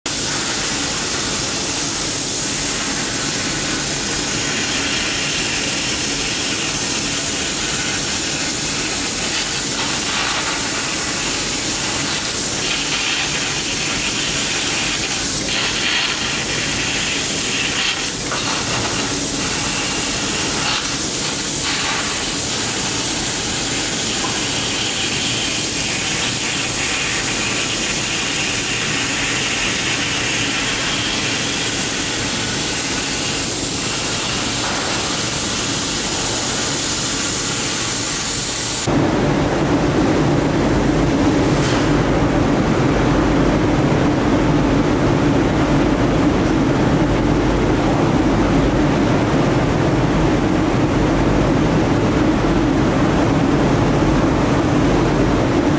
SENFENG丨Born for Ultra-high Power
20mm carbon steel, cut with Senfeng 20kW laser cutting machine.